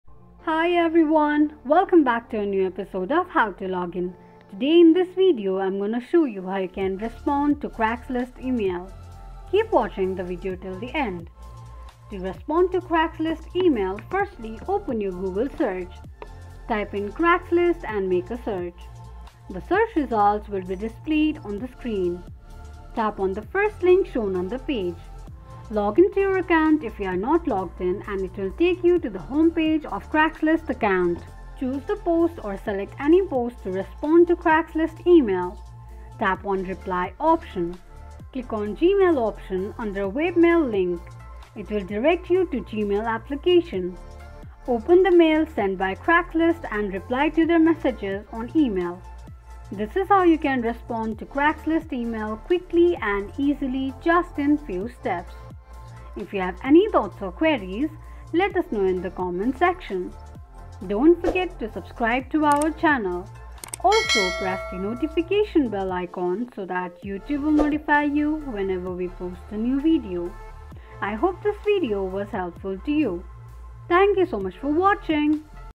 How to Respond to Craigslist Email 2023? Reply to Craigslist Emails
Discover effective ways to respond to Craigslist emails and communicate with potential buyers or sellers. Whether you're new to Craigslist or want to improve your email etiquette, this tutorial will guide you through the response process with useful tips. Join us to enhance your Craigslist communication skills!